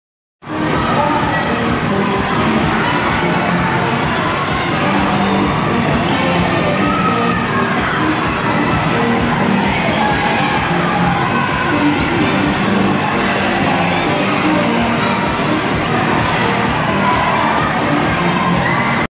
Figli vs Genitori: 100 contro 100
La partita goliardica figli vs genitori con cui si è concluso il torneo di minivolley di San Ponziano